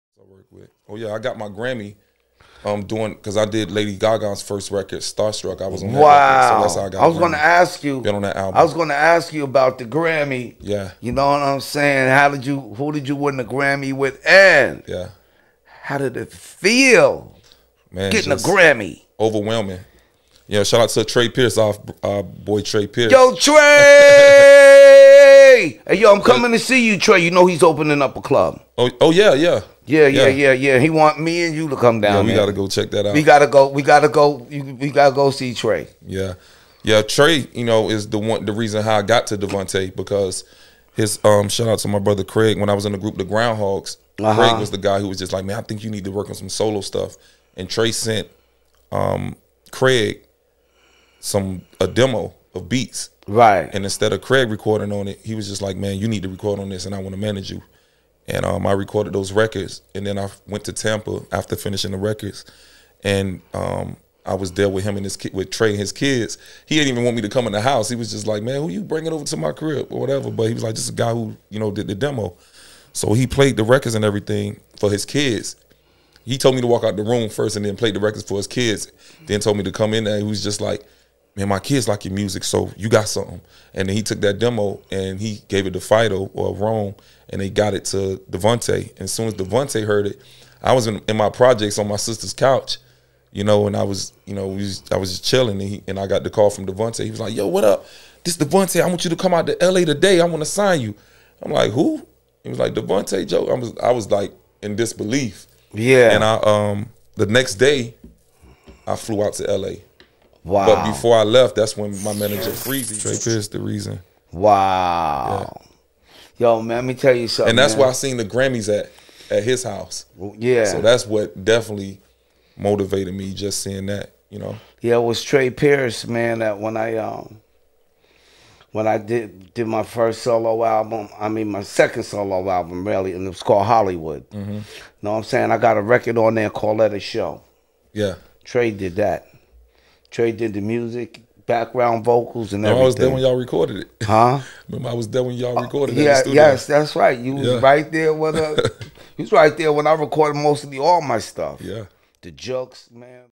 Flavor Flav, Flo Rida, talks, Trae Pierce & (Devante of Jodeci)
This Video, Flavor Flav and Flo RIda talks Grammy Awards,  friend, multi grammy winning bass player/Producer,,  Trae Pierce, Flo Rida talks of first meeting Trae and Flav talks of first working with Trae Pierce